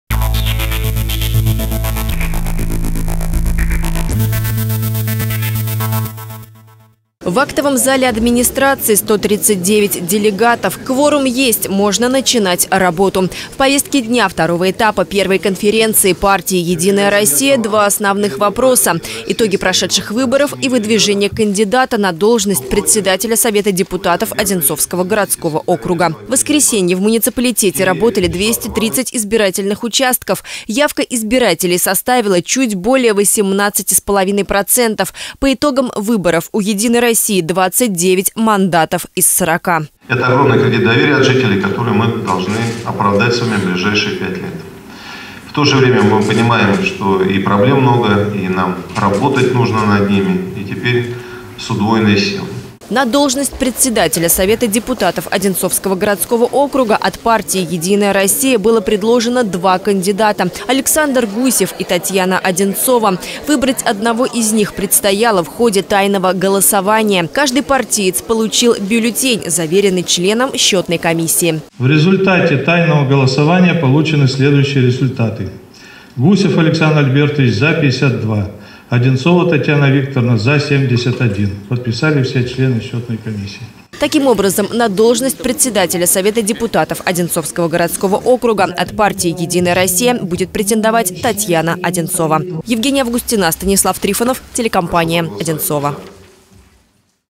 Конференция Одинцовского отделения «Единой России»
В Одинцово прошел 2-ой этап 1-ой конференции местного отделения партии «Единая Россия». Ключевой вопрос повестки – итоги минувших выборов в совет депутатов городского округа.